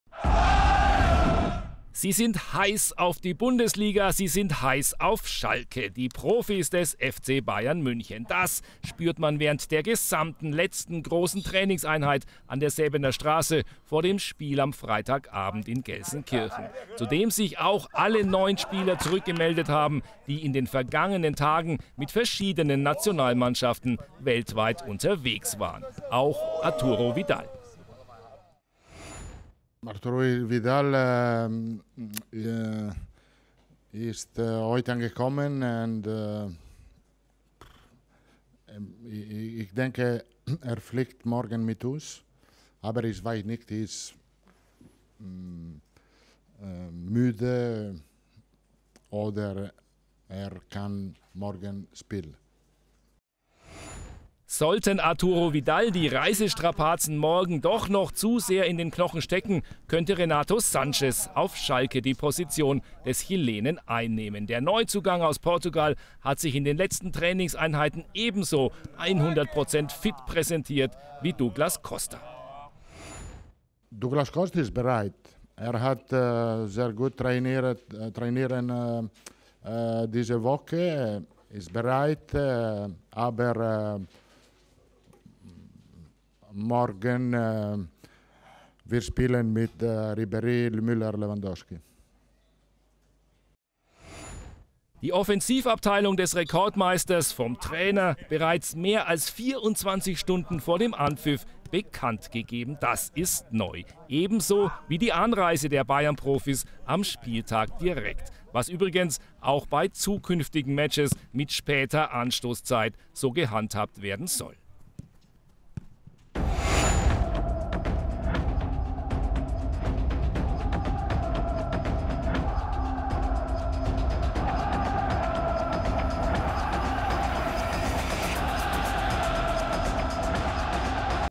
Ribéry, Müller and Lewandowski start against Schalke
Bayern are ready for Schalke: The internationals are all fit, Arturo Vidal will travel to Gelsenkirchen, Renato Sanches is ready to make his Bundesliga debut and Ribéry, Müller and Lewandowski lead the attacking line.